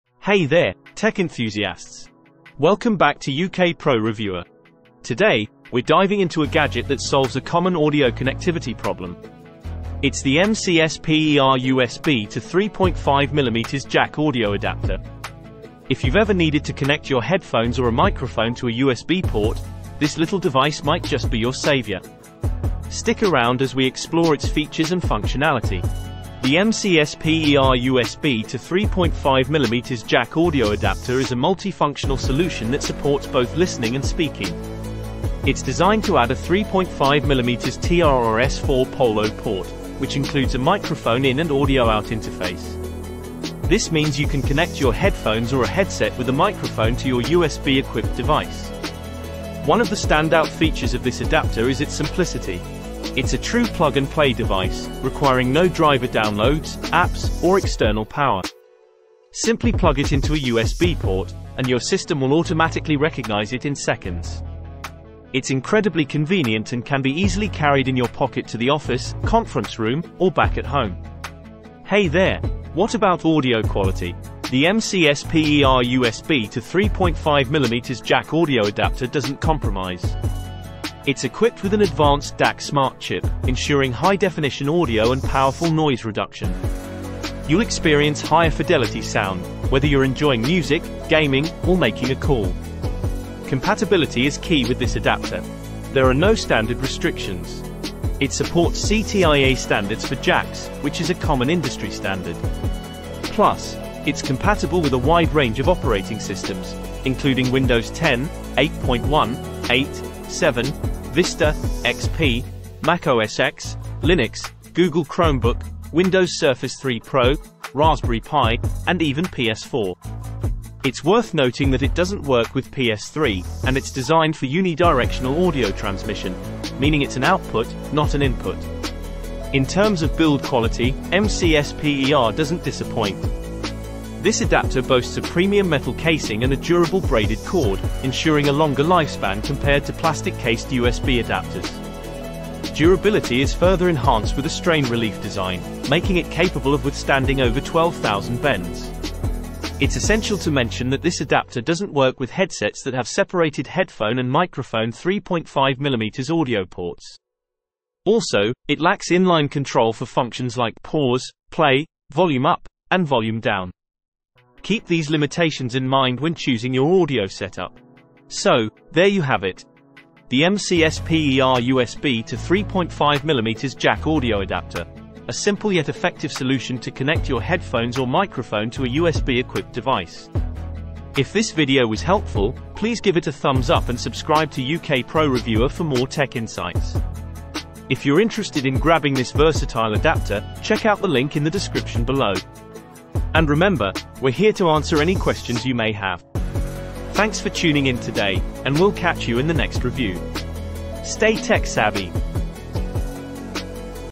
USB to 3.5mm Jack Audio Adapter,External Sound Card USB-A to Audio Jack Adapter Review
Amazon Product Name : USB to 3.5mm Jack Audio Adapter,External Sound Card USB-A to Audio Jack Adapter

Today, we're exploring the world of audio with a review of the USB to 3.5mm Jack Audio Adapter, an external sound card that converts USB-A to an audio jack. Whether you're looking to improve your audio setup or need a quick fix for connecting headphones or microphones, this review has you covered. Join us as we examine its features, functionality, and how it can enhance your audio experience. If you're in the market for a versatile audio solution, don't miss this video.